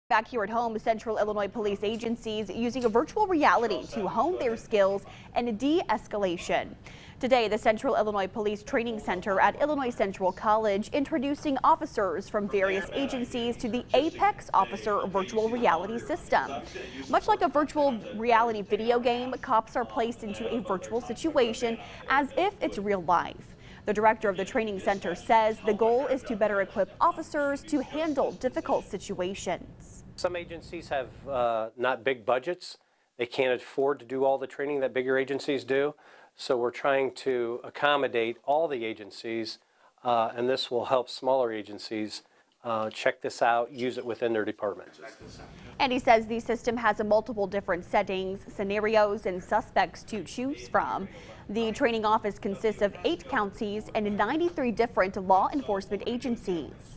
Central Illinois Police Adopt New Training Simulator
Central Illinois police agencies are now using virtual reality training to hone their skills in de-escalation. Director of Central Illinois Police Training Center Brian Fengel said the goal of the Apex Officer simulator is to prepare new officers on how to respond in difficult situations.